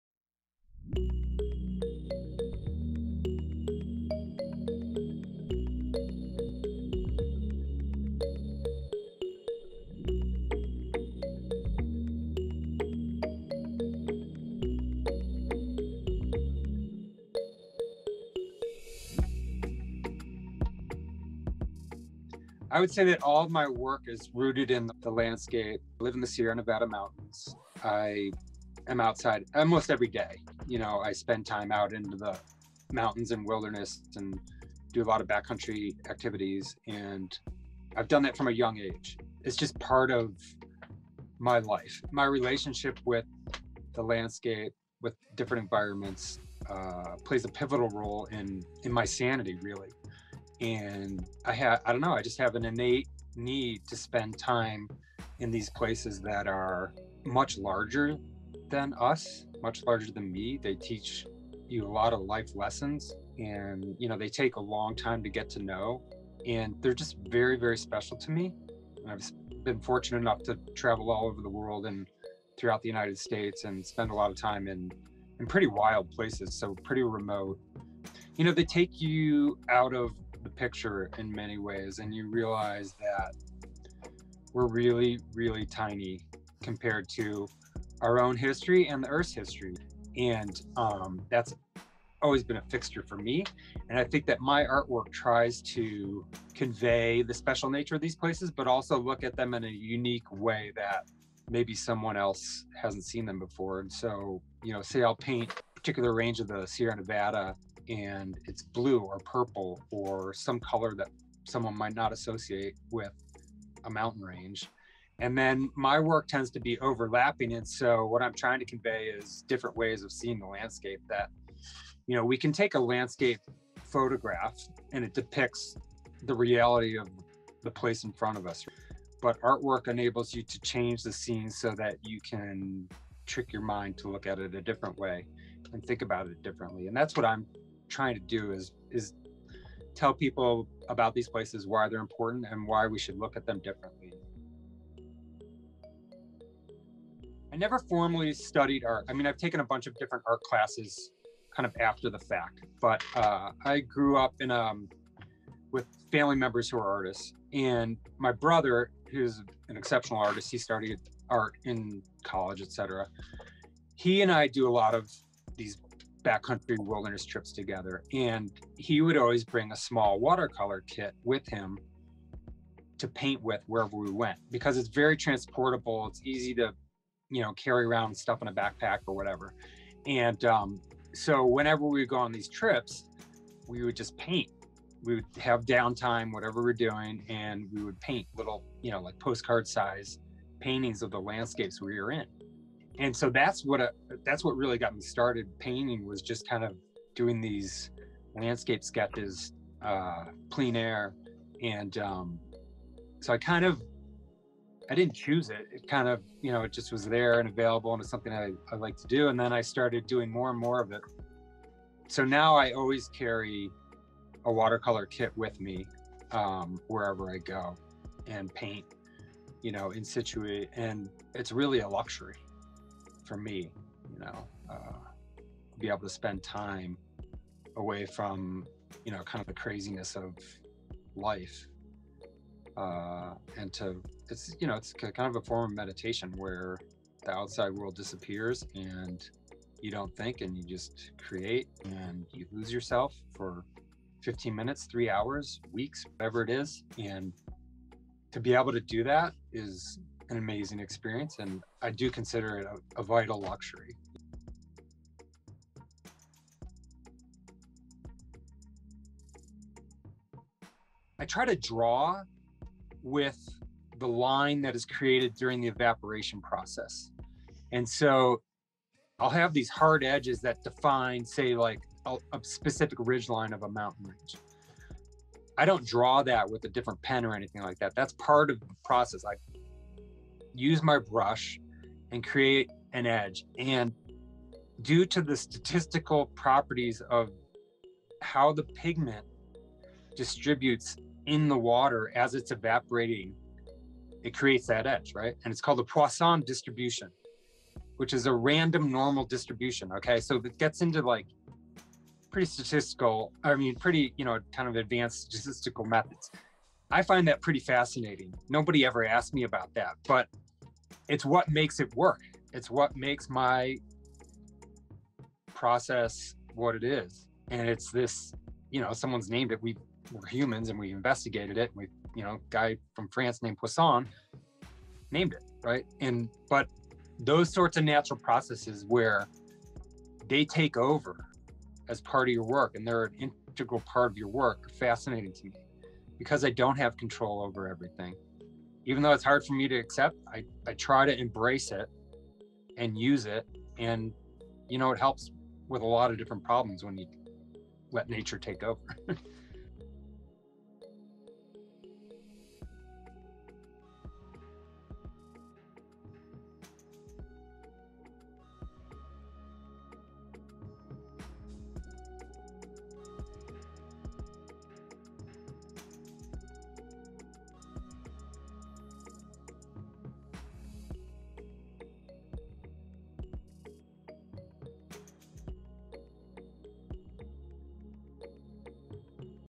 3 Questions Digital Series with Chris Crossen
An interview from Art in Embassies 3 Questions Digital Series with Chris Crossen, who speaks about his creative process and artwork at the U.S. Ambassador’s residence in Riyadh, Saudi Arabia.